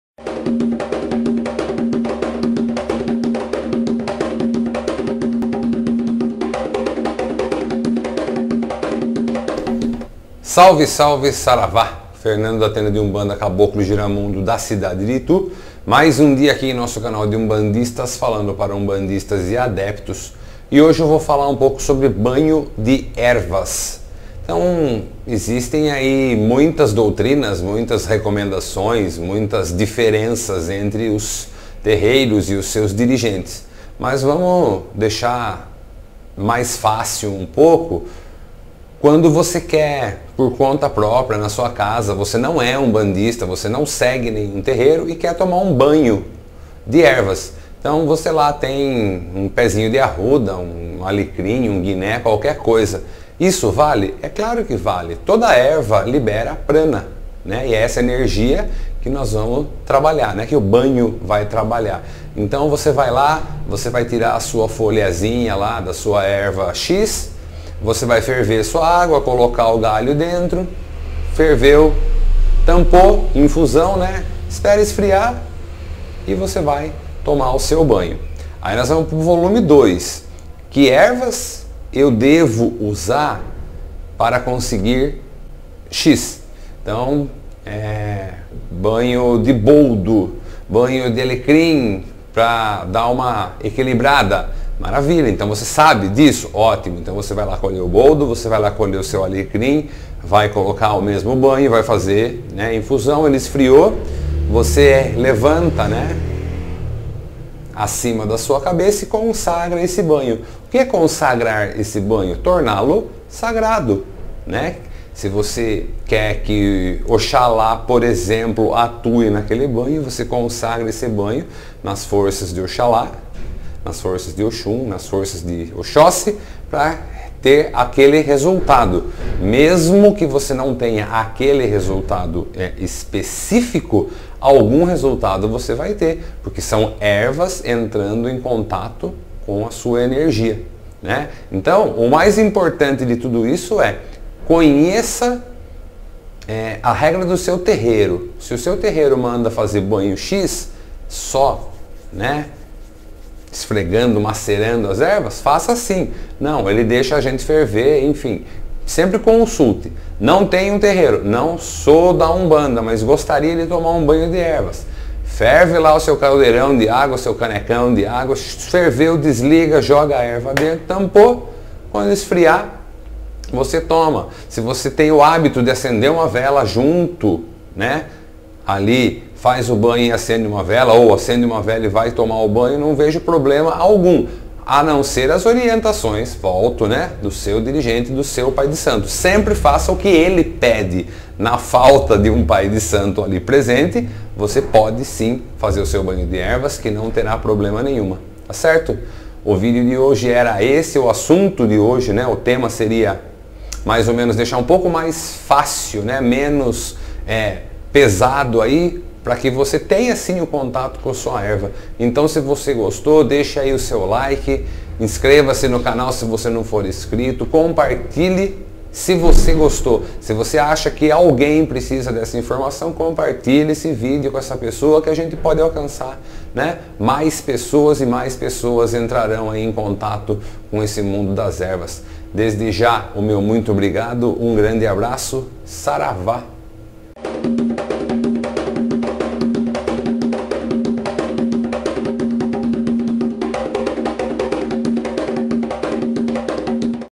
Banho com ervas
- Posso tomar banho com ervas?
- Posso usar qualquer erva em meu banho?
- Toda pessoa pode tomar banho com ervas?
- Preciso acender uma vela antes do banho?
- Banho de ervas maceradas ou infusão?
- Não sou da Umbanda, posso tomar banho com ervas?